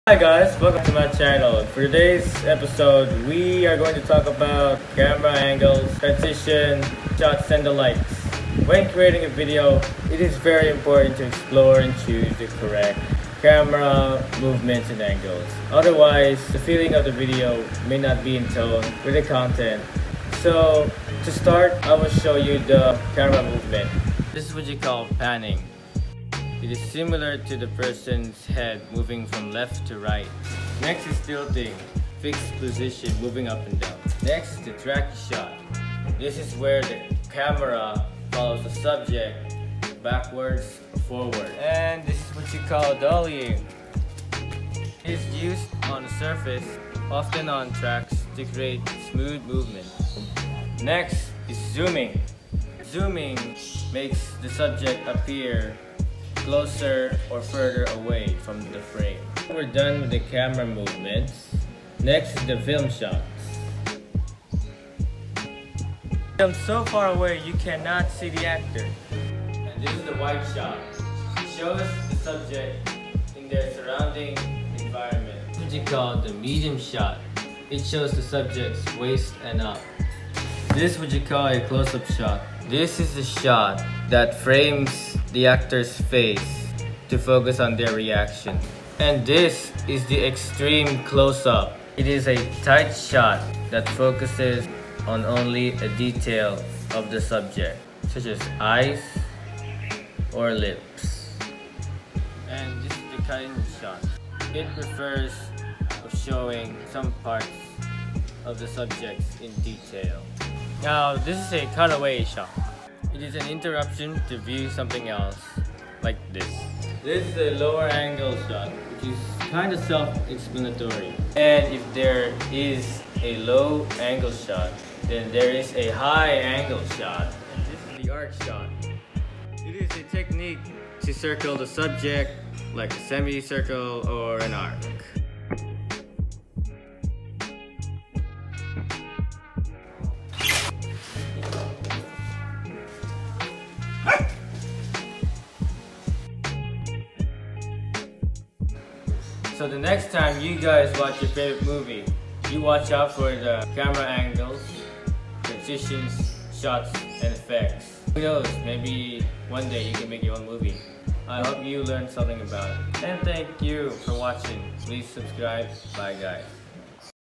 VIDEOGRAPHY 101
Hi guys! It’s me Lilai! But today my son Raven will share to you some videography tips for you to know if you plan to make your own videos in the future!

Hope you learn something from this, I did! 🥰